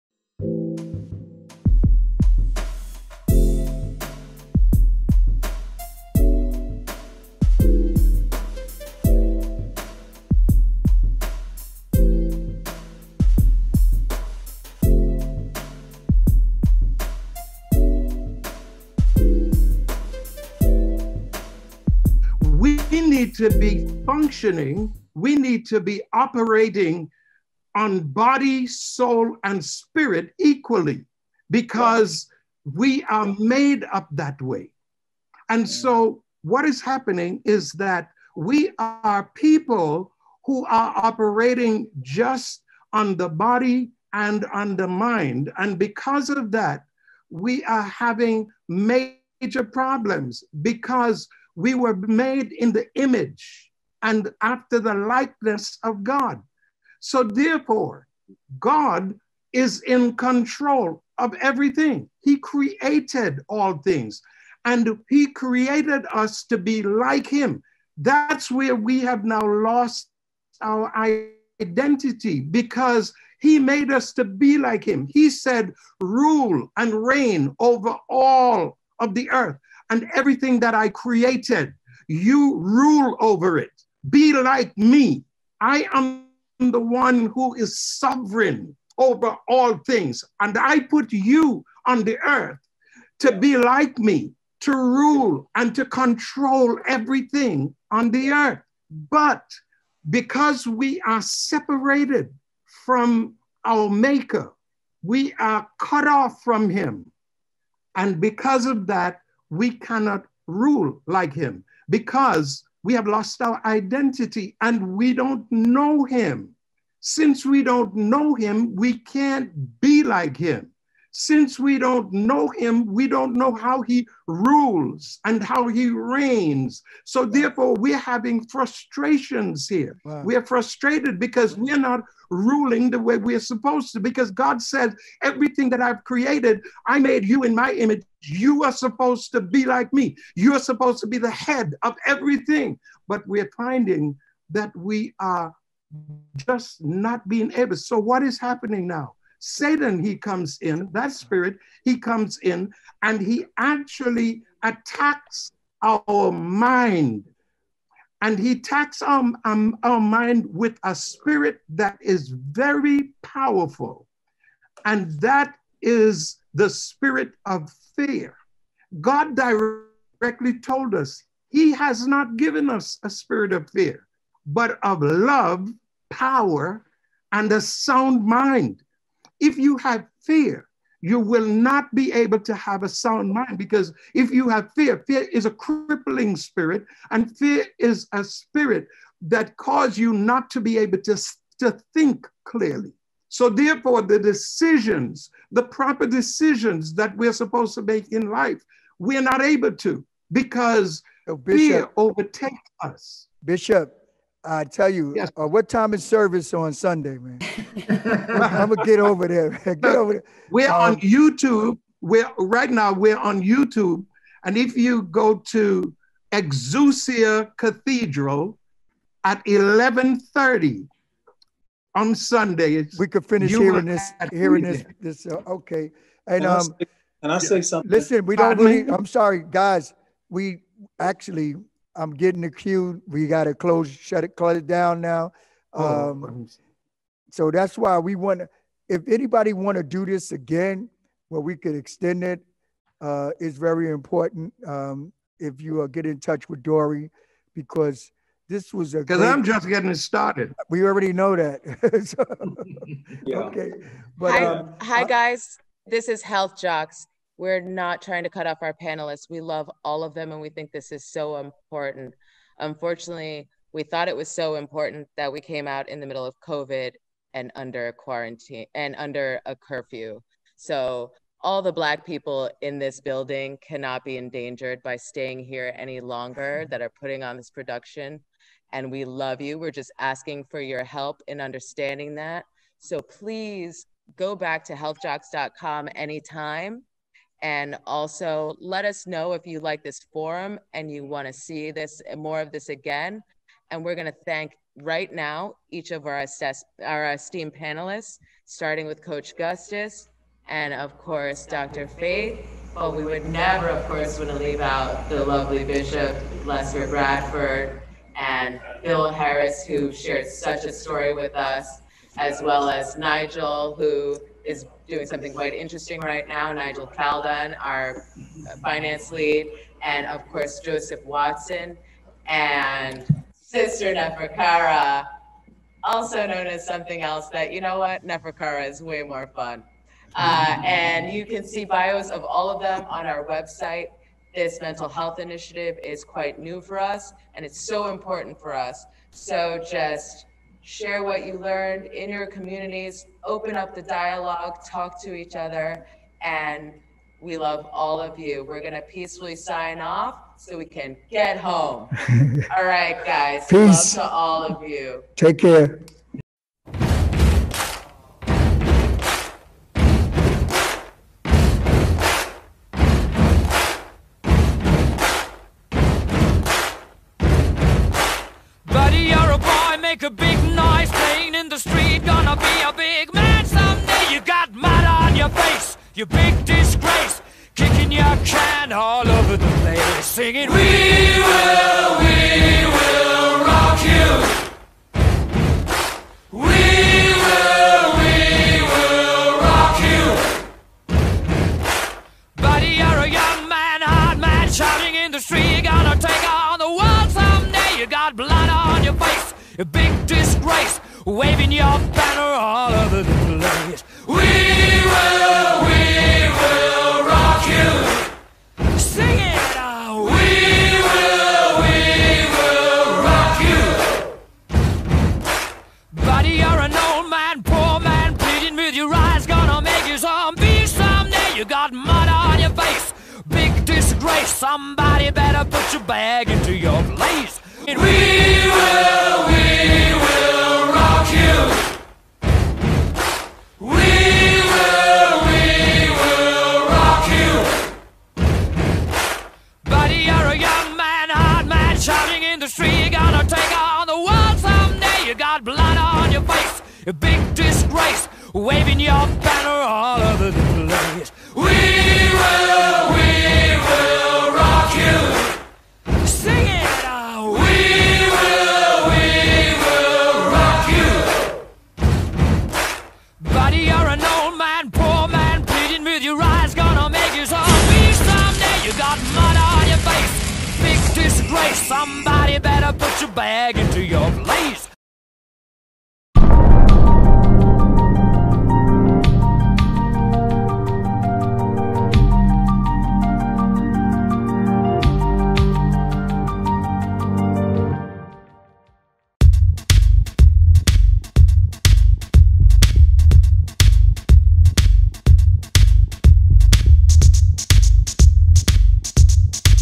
"HealthJox Virtual Town-Hall Meeting" - 2020 (Episode 007)
This is the 7th episode of the "HealthJox Virtual Town-Hall Meeting" - 2020 featuring Bishop Lester Bradford discussing the relationship between "Mind, Body & Soul".

Also Showcasing:
- Dr. Faith Brown
- Coach Ted Gustus